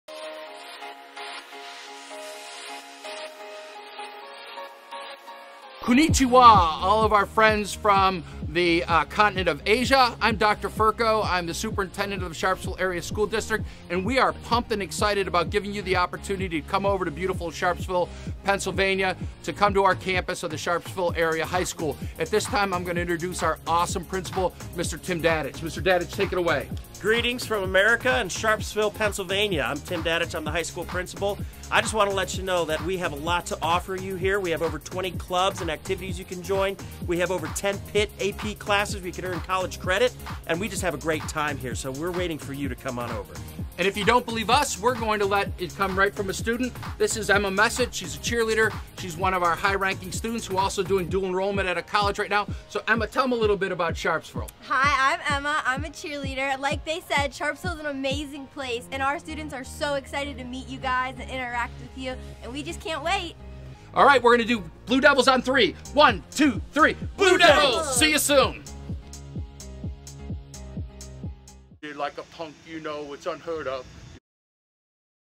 Sharpsville High School Welcomes International Students
Sharpsville School District Superintendent, Dr. Ferko and High School Principal, Tim Dadich welcomes international students from Asia, the Blue Devils way.

Sharpsville High School is a